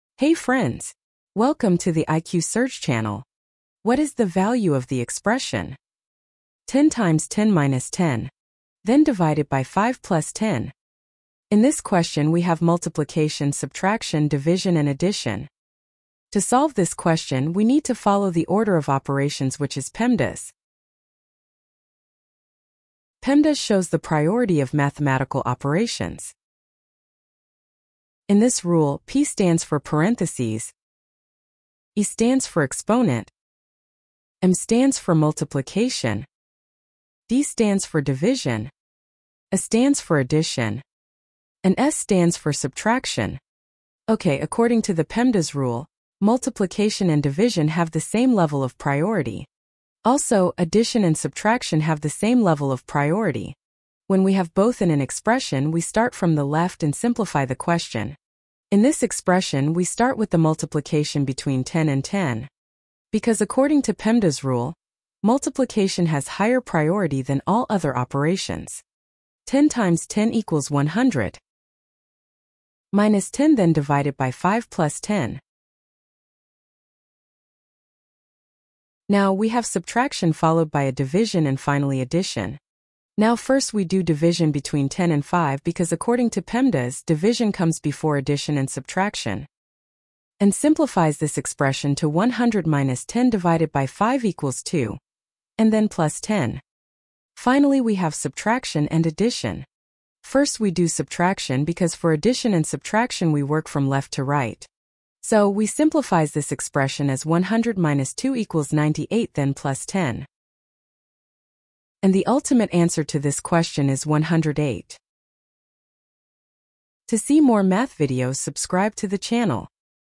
Only 1 in 10 People Can Solve This Math Problem 😱
Looks easy… but ONE PEMDAS mistake changes everything 😱
Most people rush this math problem and get it WRONG!

👉 Can you solve it before the answer?
Comment your answer 👇 and test your brain 🧠

Only 1 in 10 people follow PEMDAS correctly 🤯
Watch till the end to see the RIGHT solution!